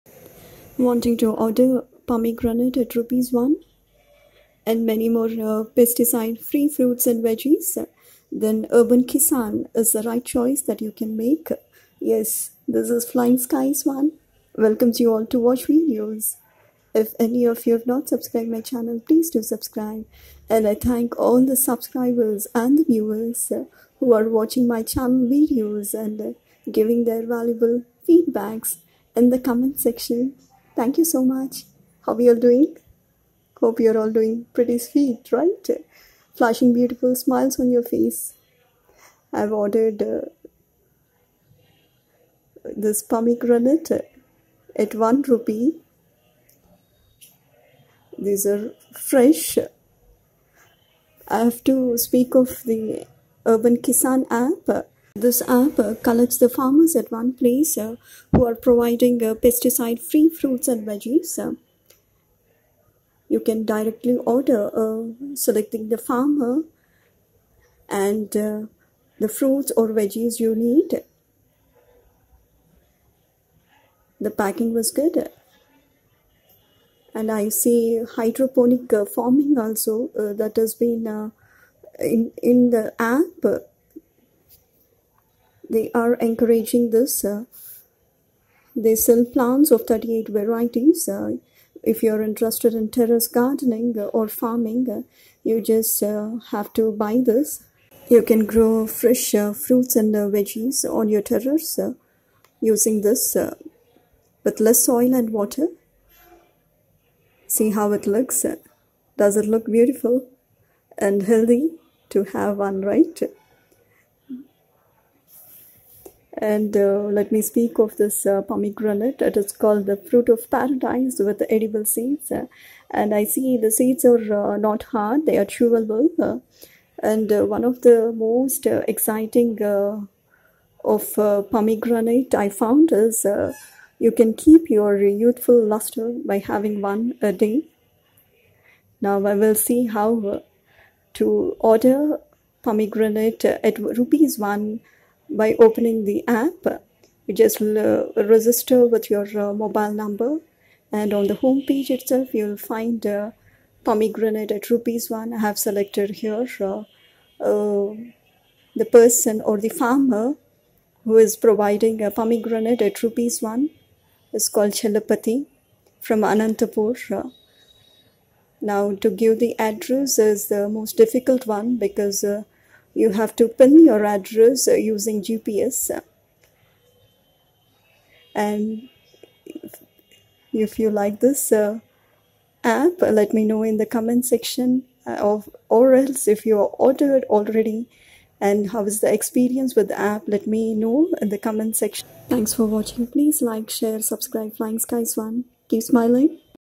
Urbankisaan first order Rs.1 unboxing&review|how to order chemical free fruits&veggies from farmers
Purchased pomegranate kg at rs.1 on urbankisaan app , No minimum order, delivery charges applicable if it's cash on delivery. .
* to login Please register with your phone number
*Add your address, using 'locate GPS' by clicking on account tab
*select premium pomegranate kg Rs1 or any product at Rs.1 available right on home page
*delivery charges applicable according to your area' ORDER using COD option
*After placing order, you can cancel then and there according to the time prescribed.

Please Note- COD not availabe for some 1re. orders. No tracking ID is available, but mail and sms is received for the confirmation of orders placed.

For concerns of wrong or no products received- go to 'help' in past orders or  seek 'chat' option on app

अर्बनकिसान ऐप पर 1 रुपये में अनार किलो खरीदा, कोई न्यूनतम ऑर्डर नहीं, डिलीवरी शुल्क लागू। कृपया अपना पहला ऑर्डर रु.1 पर देना न भूलें। गूगल प्ले स्टोर से ऐप डाउनलोड करने के लिए यहां क्लिक करें
*लॉगिन करने के लिए कृपया अपने फोन नंबर के साथ रजिस्टर करें
*खाता टैब पर क्लिक करके 'लोकेट जीपीएस' का उपयोग करके अपना पता जोड़ें
* होम पेज पर उपलब्ध प्रीमियम अनार किलो 1 रुपये या किसी भी उत्पाद का चयन करें
*आपके क्षेत्र के अनुसार लागू डिलीवरी शुल्क' COD विकल्प का उपयोग करके ऑर्डर करें
*आदेश देने के बाद, आप निर्धारित समय के अनुसार तब और वहां रद्द कर सकते हैं।

कृपया ध्यान दें- COD कुछ 1re के लिए उपलब्ध नहीं है। आदेश। कोई ट्रैकिंग आईडी उपलब्ध नहीं है, लेकिन दिए गए आदेशों की पुष्टि के लिए मेल और एसएमएस प्राप्त होता है।

गलत या कोई उत्पाद प्राप्त नहीं होने की चिंताओं के लिए- पिछले आदेशों में 'सहायता' पर जाएं या ऐप पर 'चैट' विकल्प की तलाश करें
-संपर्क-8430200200

 keep smiling,😄